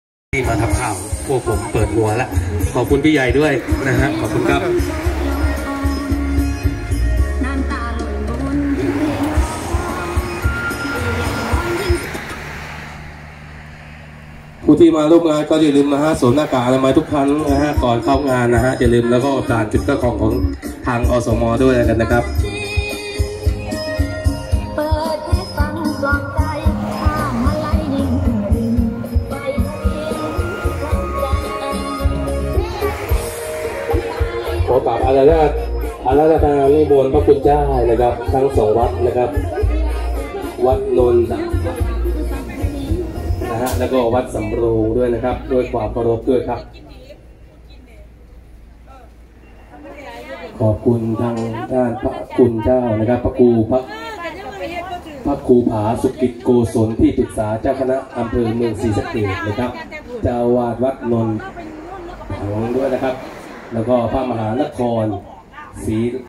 พิธีบวงสรวงศาลหลังใหม่ ฮันนี่ ศรีอีสาน ศาลเจ้าแม่ฮันนี่ ศรีอีสาน ถนน 226 ก่อนเข้าเมืองศรีสะเกษ
พิธีบวงสรวงศาลหลังใหม่ ฮันนี่ ศรีอีสาน เจ้าแม่ฮันนี่ ศรีอีสาน ถนน 226 ก่อนเข้าเมืองศรีสะเกษ 26 กพ. 2565